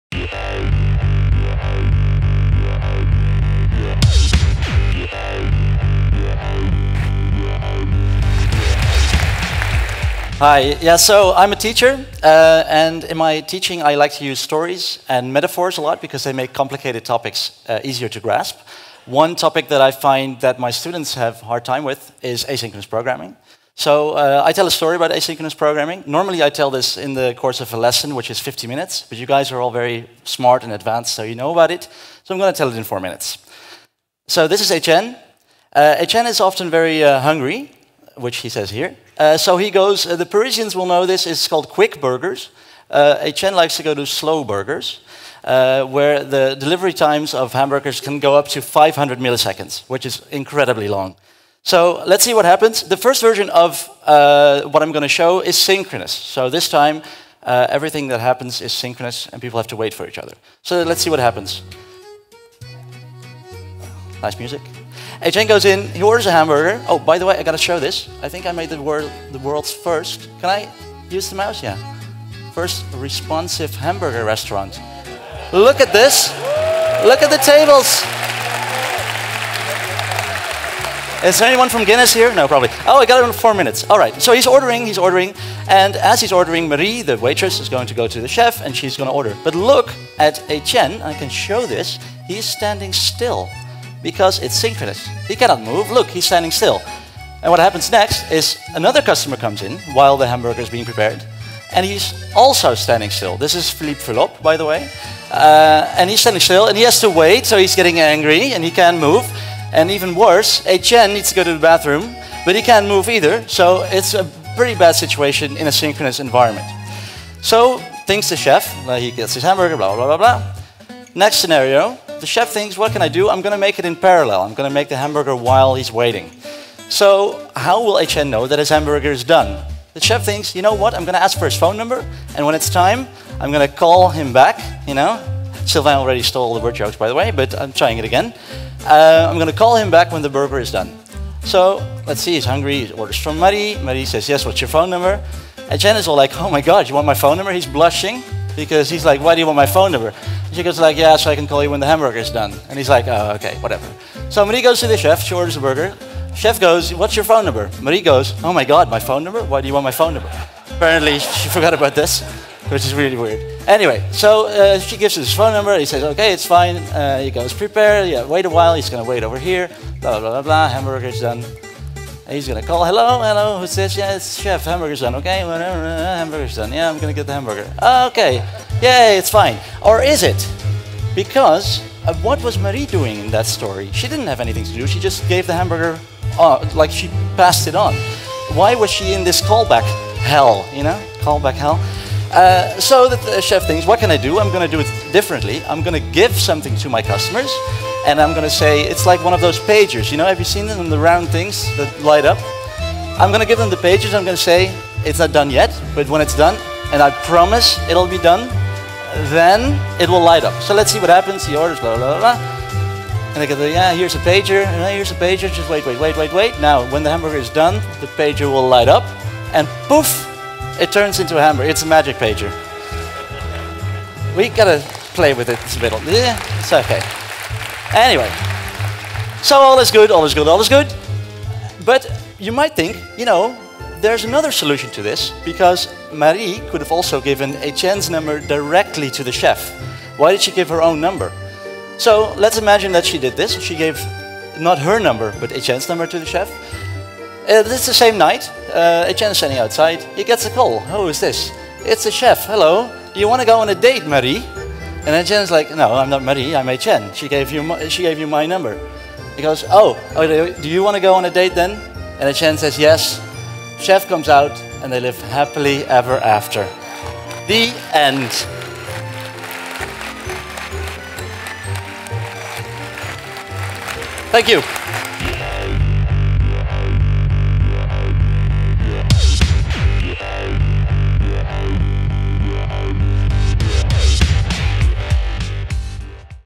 dotJS 2018 - Joost Lubach - Asynchronous hamburgers
on November 9th in Paris.

While teaching, Joost finds that his beginning JS students have a hard time grasping the concept of asynchronous programming. So he tells a story about Étienne who orders a hamburger from Slow Burgers, where prep times may be as crazy as *500ms*.

Through an animated cartoon environment, Joost shows various sync and async concepts.